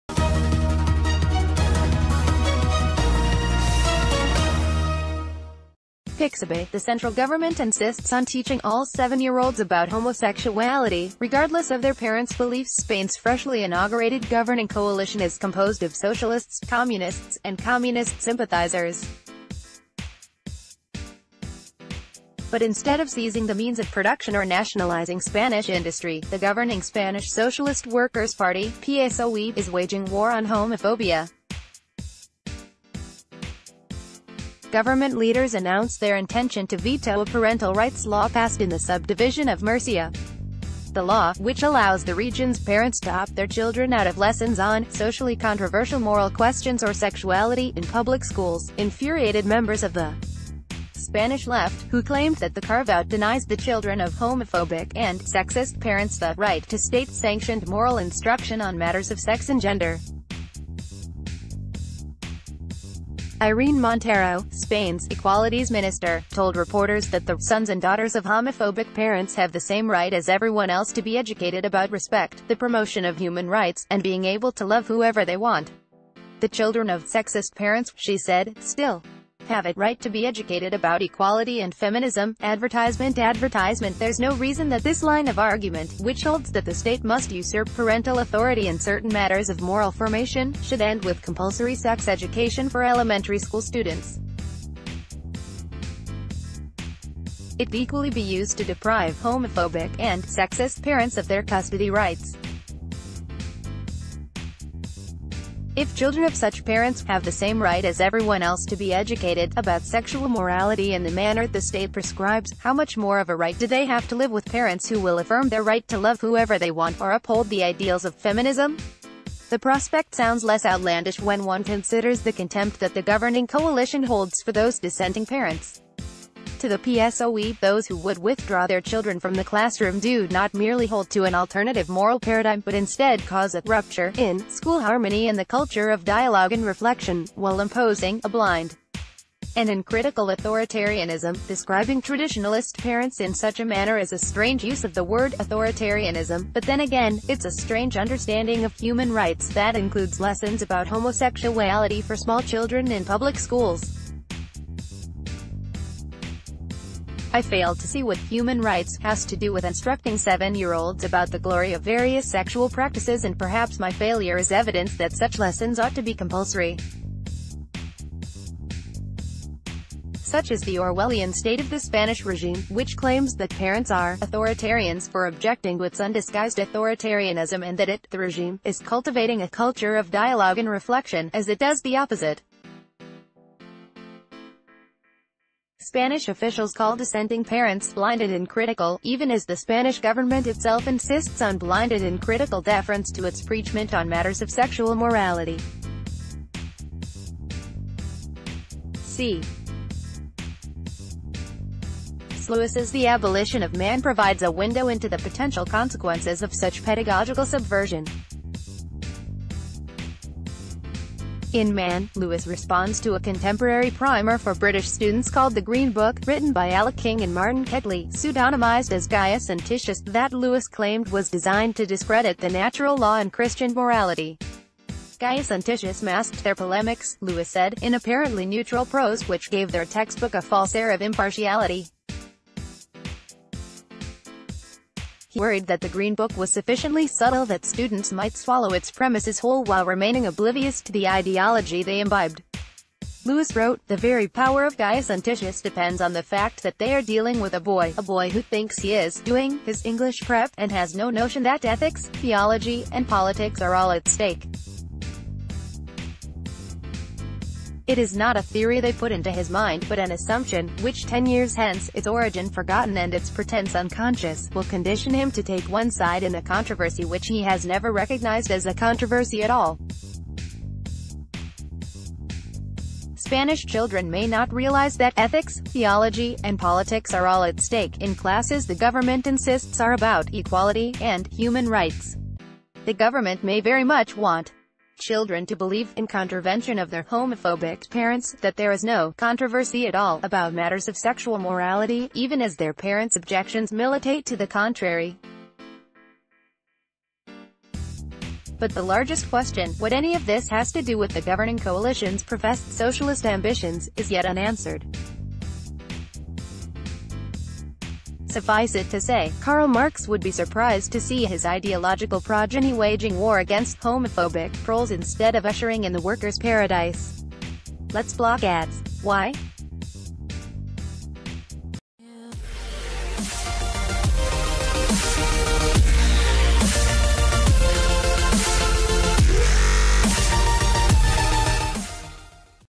In Spain, Compulsory Sex Education for Primary-School Children | National Review
(Pixabay) The central government insists on teaching all seven-year-olds about homosexuality, regardless of their parents’ beliefs Spain’s freshly inaugurated governing coalition is composed of socialists, Communists, and Communist sympathizers. But ...